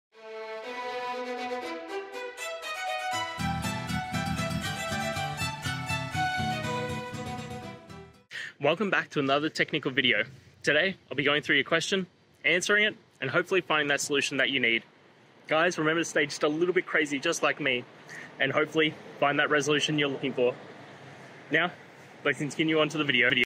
Angular 4 Component ngOnInit not called on each route request
angular: Angular 4 Component ngOnInit not called on each route request

Related to: angular, angular2-routing, angular2-components